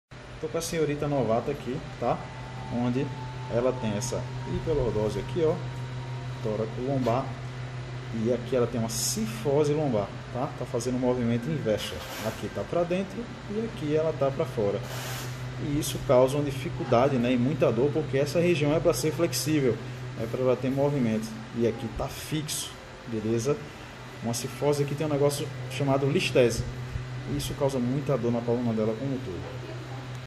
Listese Lombar
Tratamento para prevenção e melhora dos sintomas é muitoooo importante para trazer a sua vida de volta.

Precisando de Fisioterapia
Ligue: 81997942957

Fisioterapia para músicos